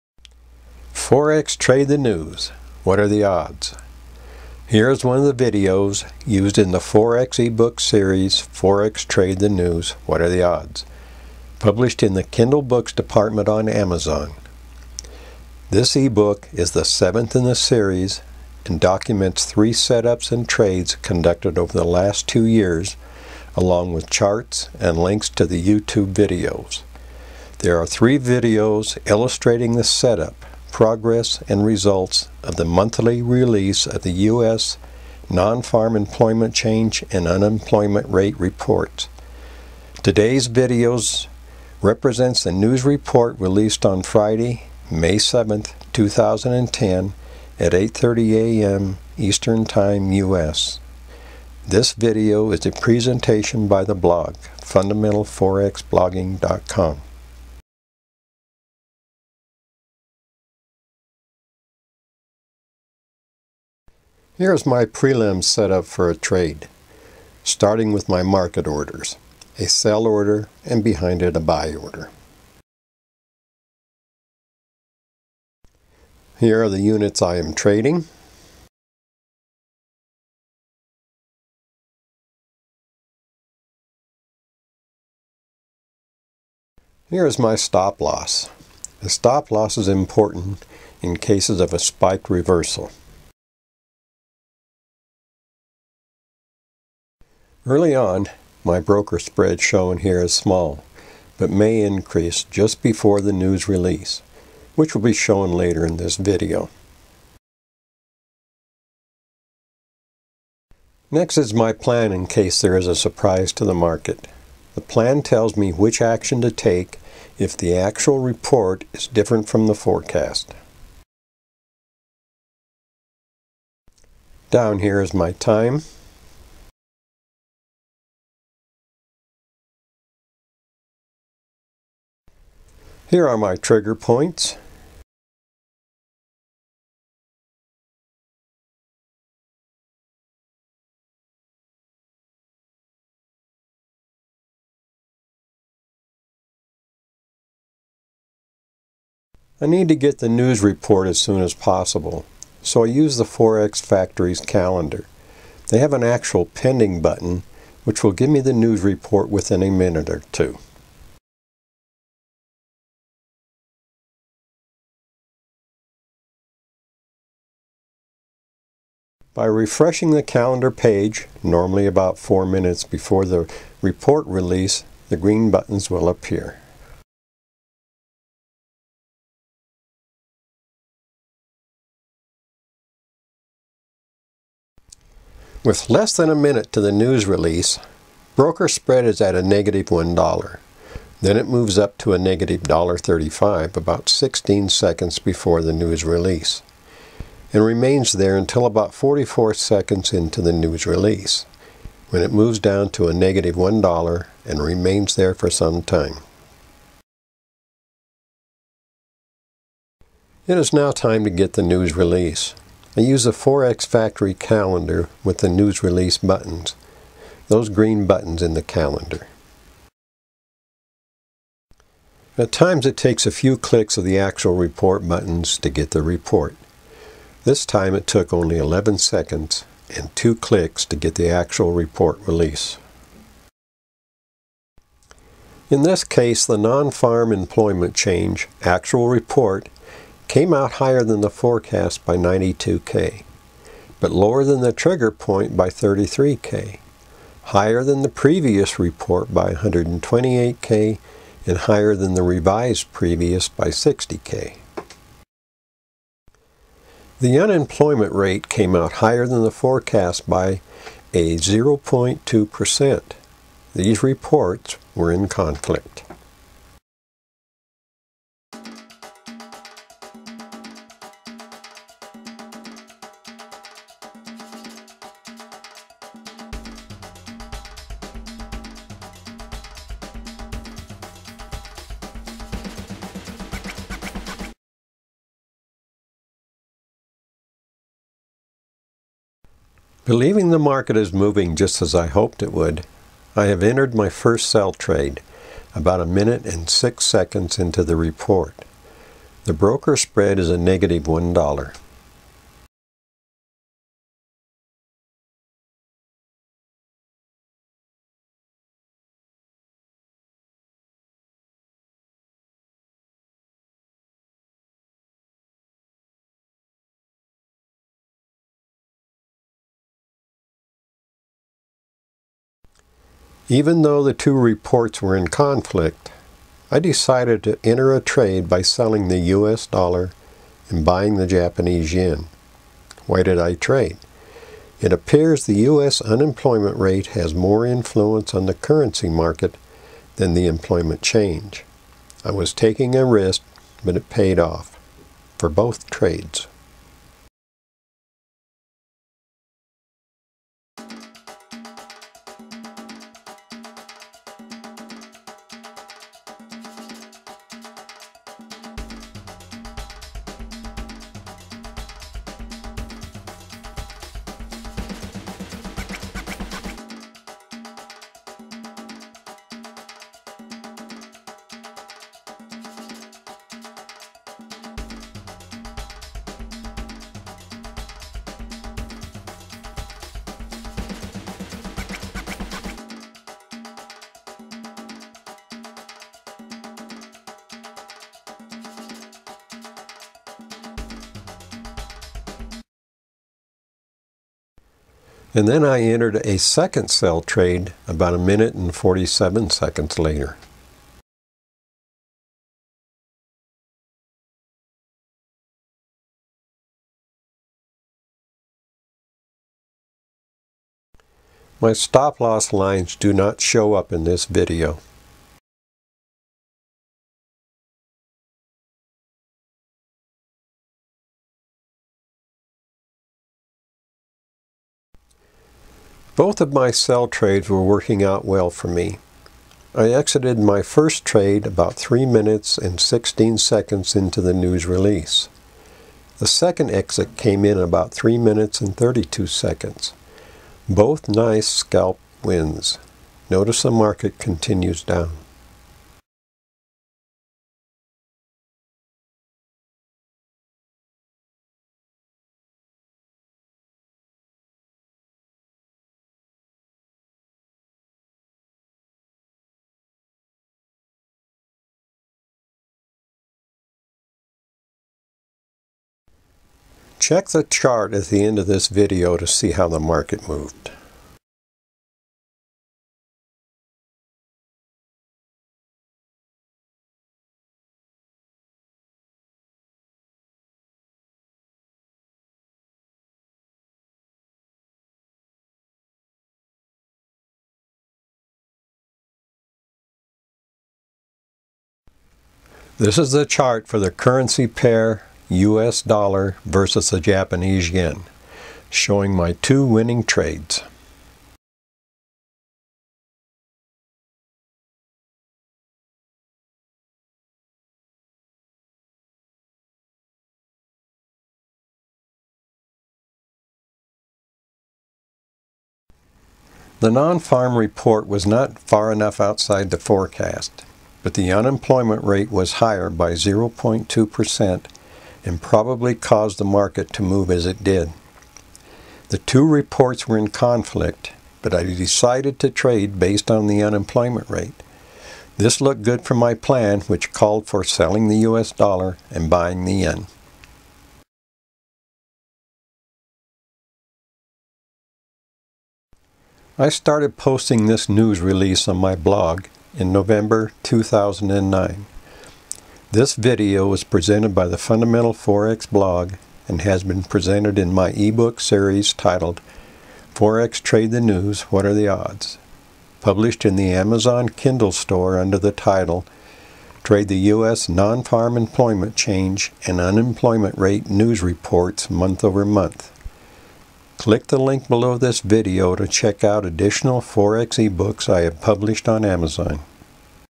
FOREX Trading the US Non-Farm Employment Change-Unemployment Rate reports for 05-07-10
What are the odds trading currency pair USD/JPY during the release of the US Non-Farm Employment Change and Unemployment Rate Reports for the month of May, 2010